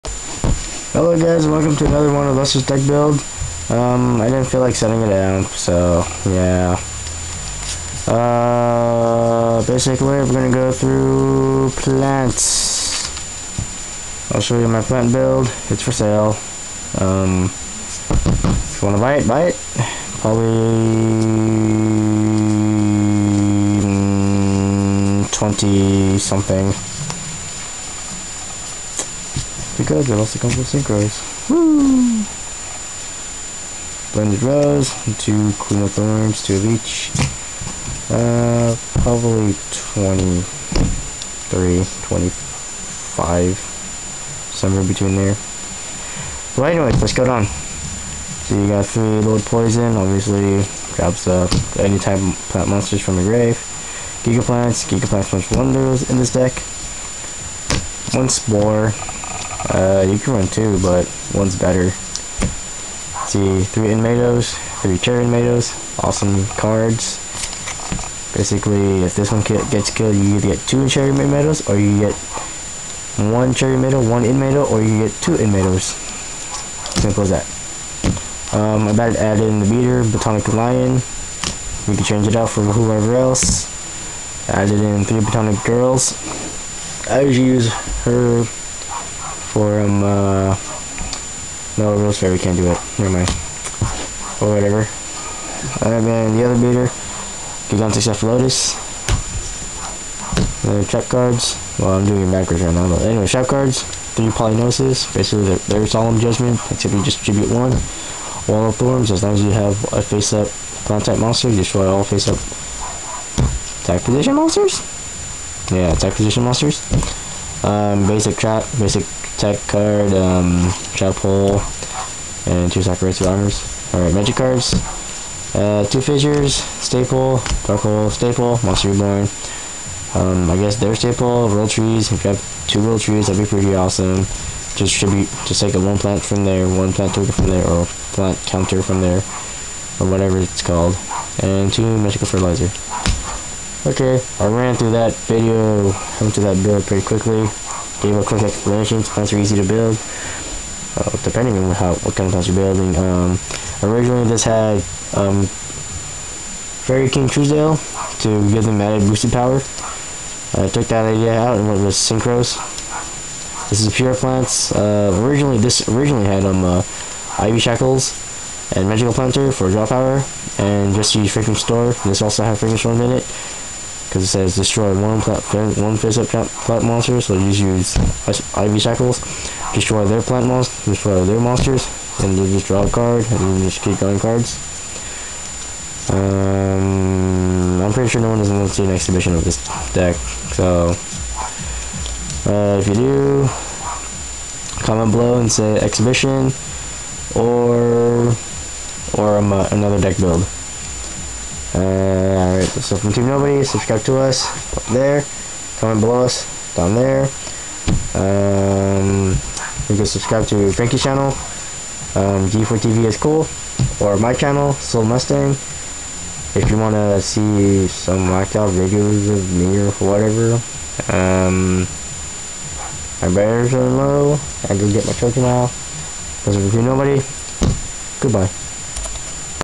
Plant Synchro - budget build
i dont use much of the higher tech cards im just using what i have.